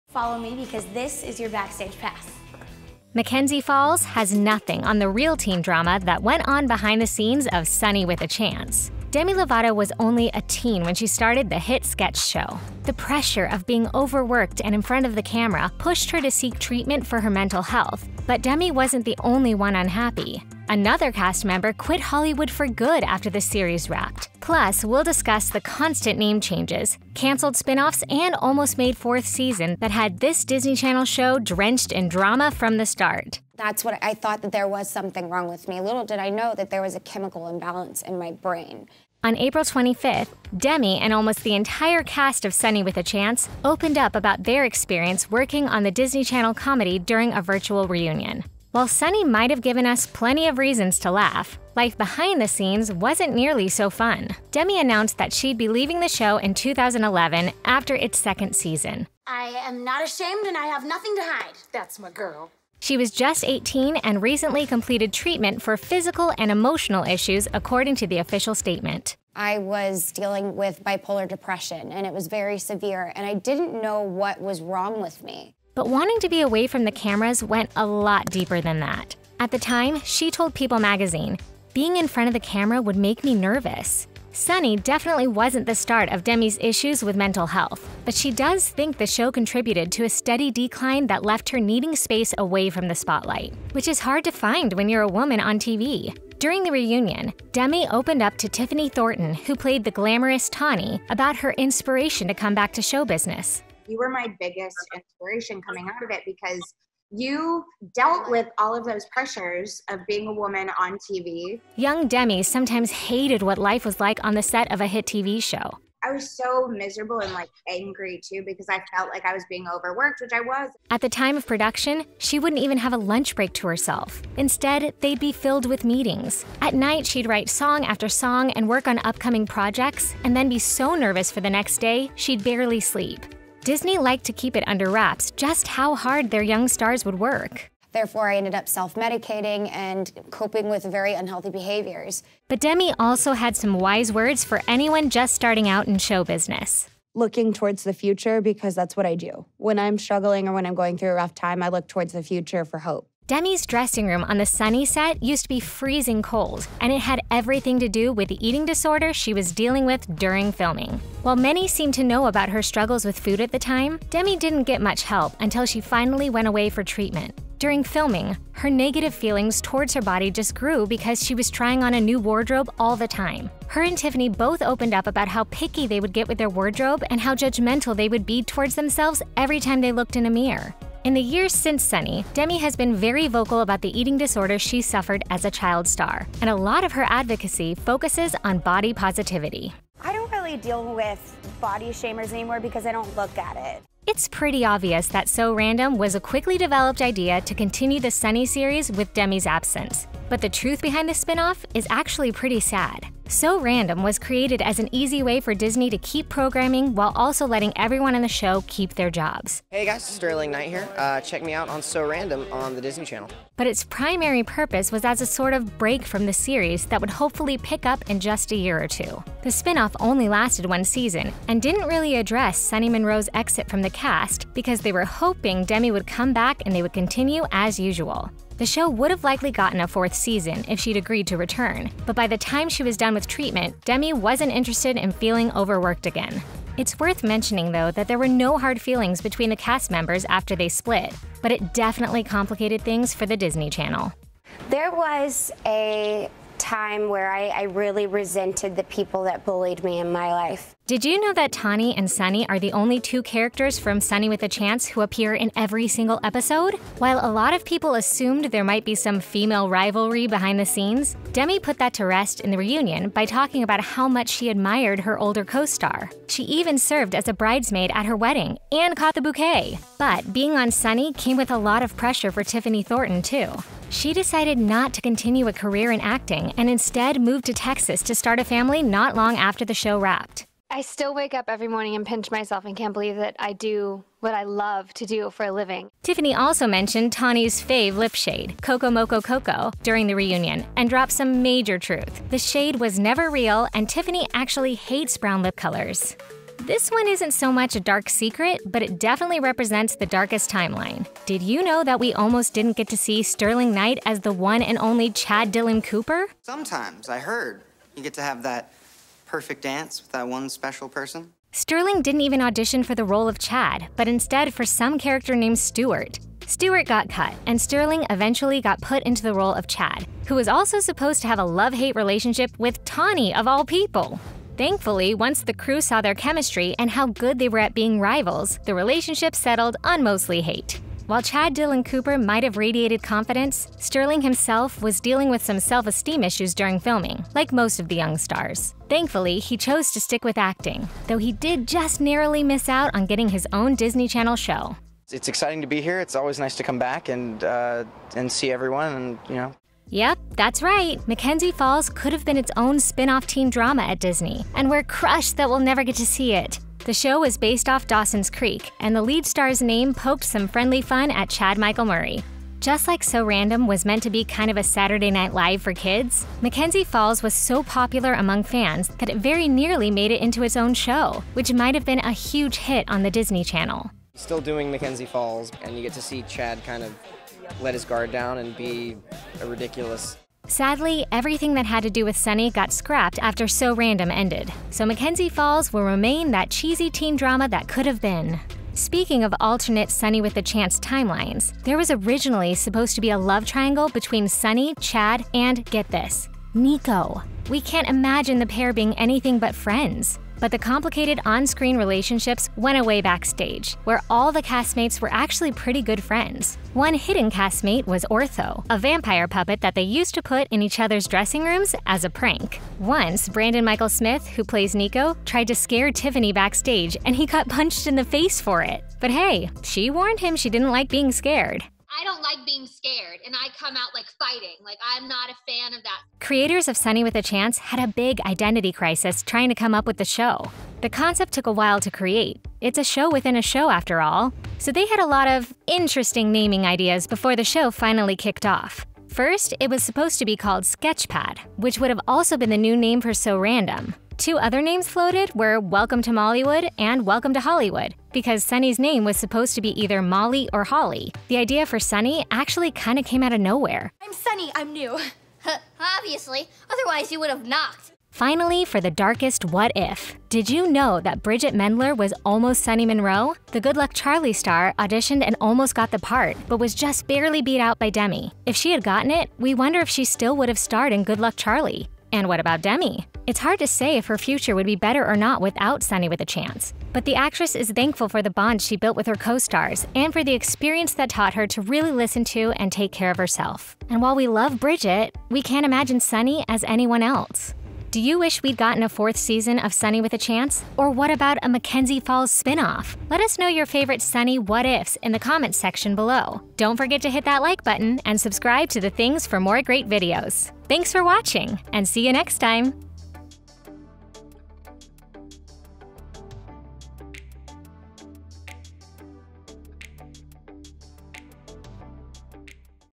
The Real Reason Demi Lovato Left Disney
Dark Secrets In Sonny With A Chance That Disney Tried To Hide

Disney's “Sonny With a Chance” was an instant hit, but after only 2 seasons Demi Lovato left the show to get help for physical and emotional issues. It’s no surprise that life starring on a teen TV show can be demanding, but the set of “Sonny” was on a whole other level. In a recent virtual reunion, the entire cast opened up about some backstage secrets, including how Demi felt overworked, was dealing with major self-esteem issues from having to always be on camera, and how she struggled with her eating disorder on set. Other stars like Tiffany Thornton and Sterling Knight also struggled with self-esteem issues. One cast member even left the spotlight for good after the series wrapped.

It wasn’t all dark and gloomy, though. The castmates love each other, and even Demi agrees she wouldn’t take back her time on the show. But with constant name changes, cancelled spin-offs, and a quickly developed band-aid for the series in the form of “So Random!”, the hit sketch show was full of issues. The show is also cursed with many what-ifs. “Mackenzie Falls” was almost a very real TV show, there was another major Disney star who almost got the role of Sonny, and there was a strange love-triangle that just barely got cut from the series. Check out our video for some of the darkest secrets from one of Disney’s brightest shows, and stay tuned until the end for some of those what-ifs that would have made “Sonny With a Chance” completely different.

Including:
Demi’s Departure
Overworked
Underfed
So Random!
Girl Power
All About Chad
Mackenzie Falls
Three’s a Crowd
Sketchpad!
Sonny’s Someone Else

Written by: Courtney Hayes
Narrated by: Tayler M
Edited by: V.Serhii